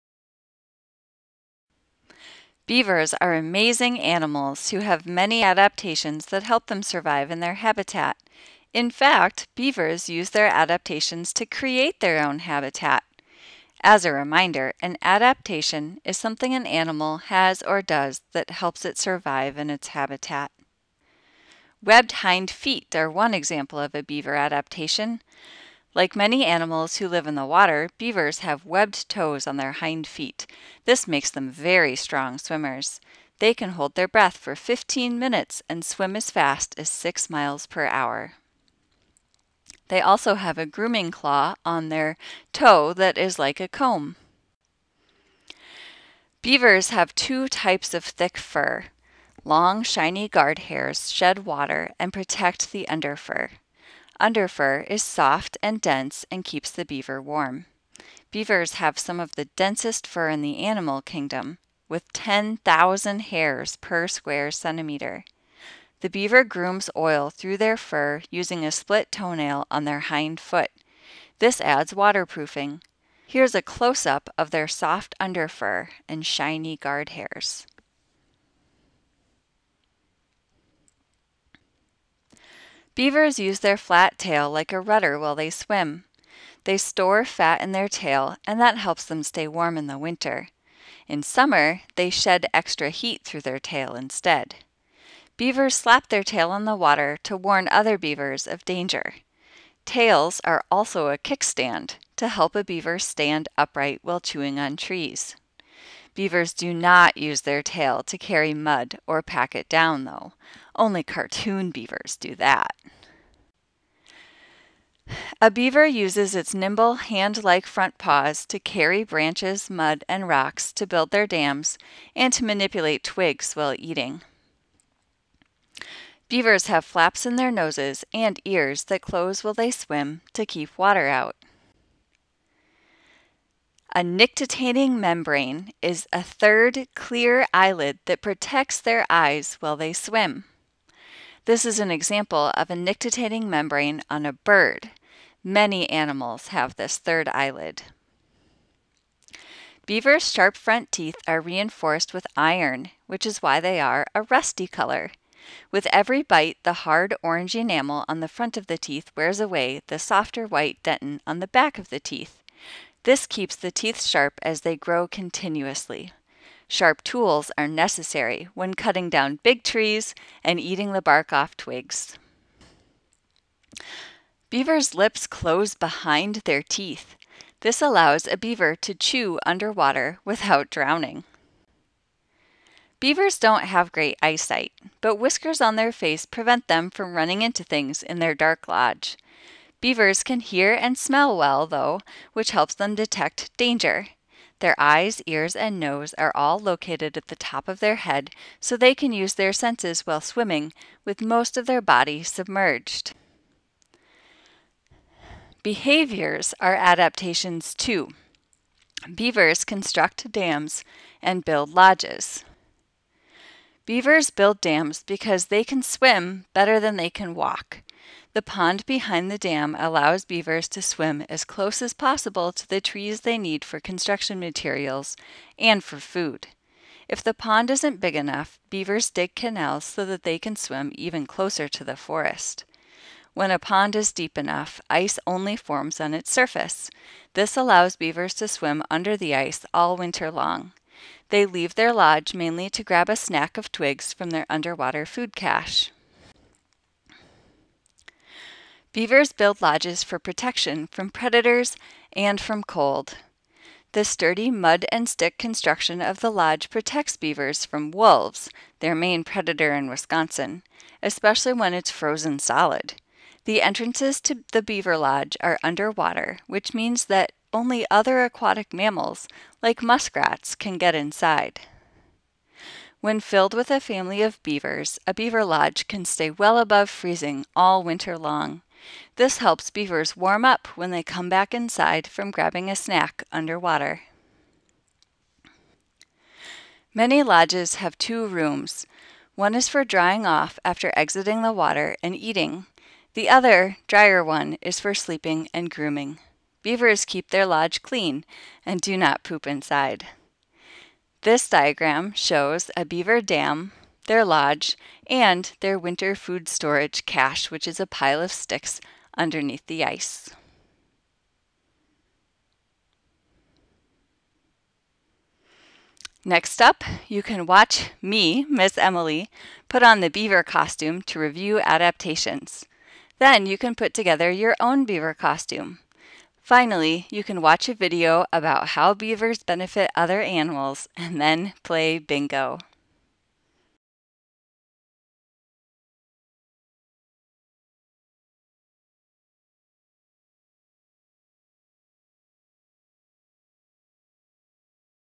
Amazing Beaver Adaptations
This video describes the amazing adaptations of North American beavers.

This video is part of the spring MuseumMobile lesson, "Amazing Beavers."